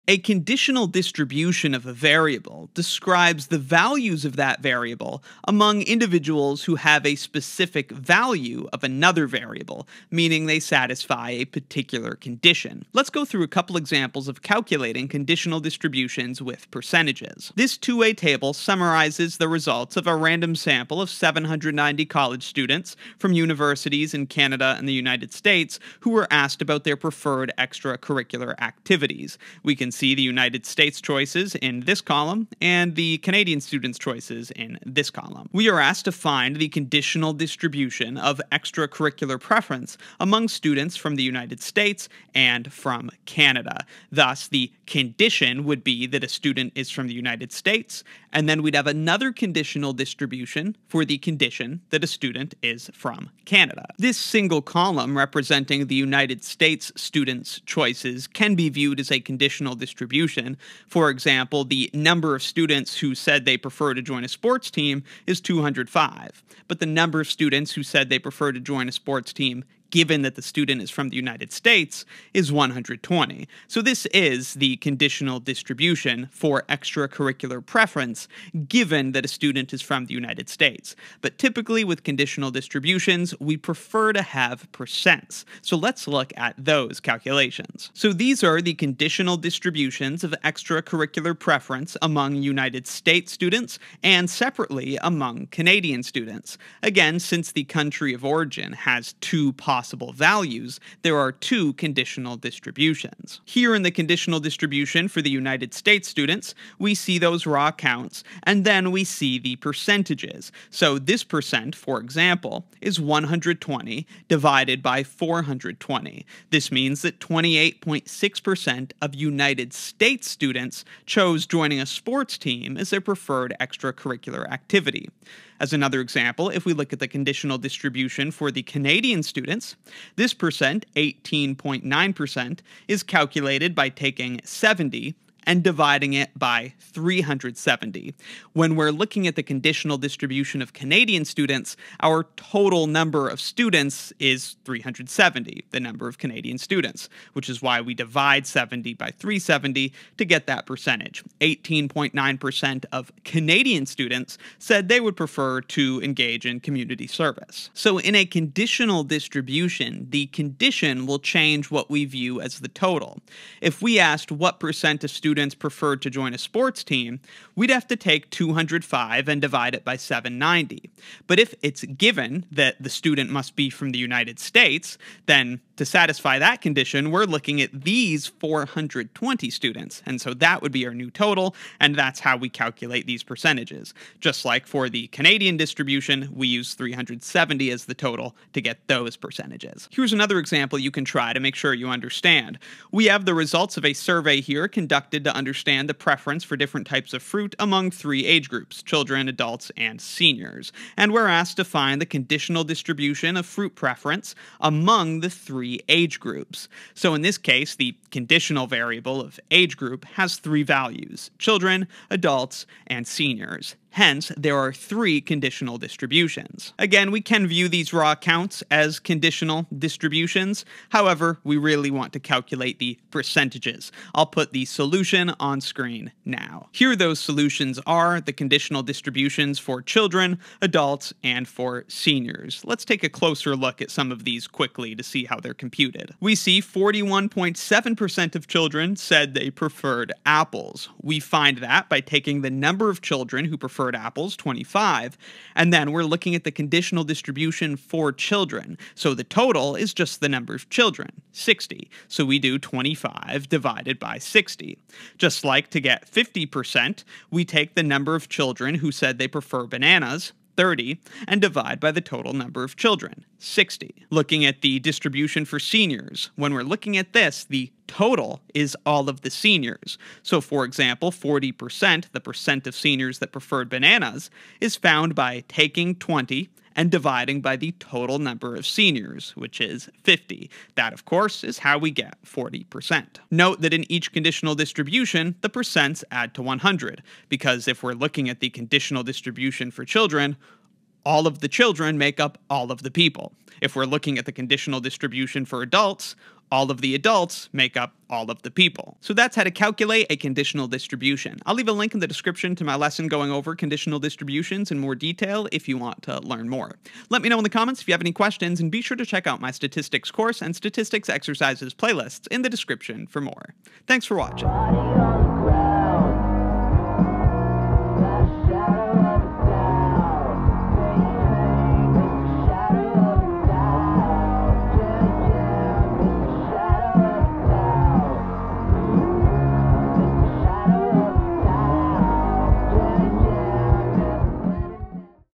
How to Find a Conditional Distribution | Statistics Exercises
Wee go over how to find a conditional distribution of a categorical variable across values of some other categorical variable. We'll see that each value of the conditional variable gives us a new conditional distribution, where the 'who' or total number of individuals has changed to reflect the condition in place. We'll do two examples of finding conditional distributions with percents.